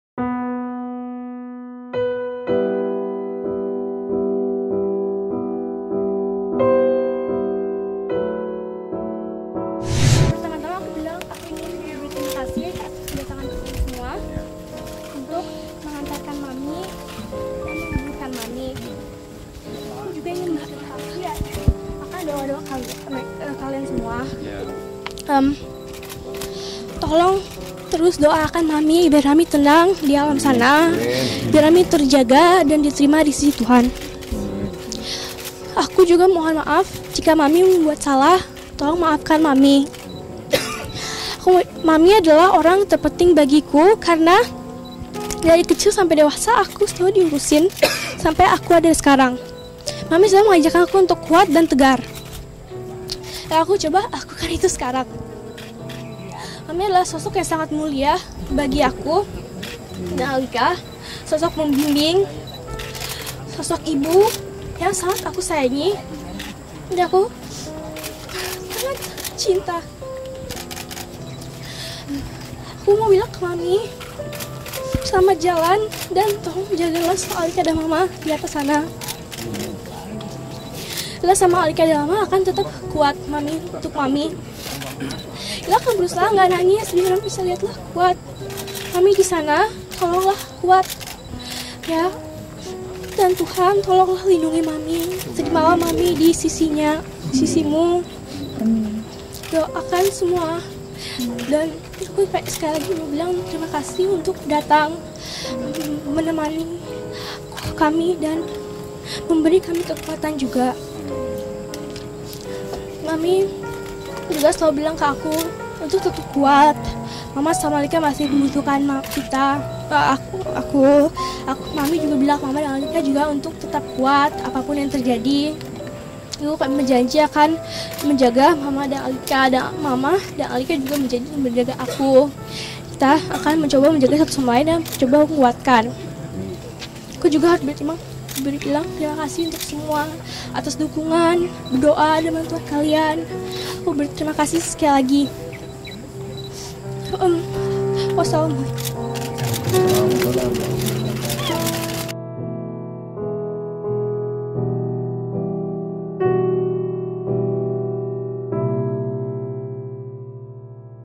Pernyataan Putri Pertama Yana Zein Usai Pemakaman
Jumat (02/06) sore, jenazah Yana Zein dimakamkan di TPU kawasan Cinere, Jakarta Selatan. Usai proses pemakaman, putri pertama Yana Zein, Dila memberikan pernyataaan.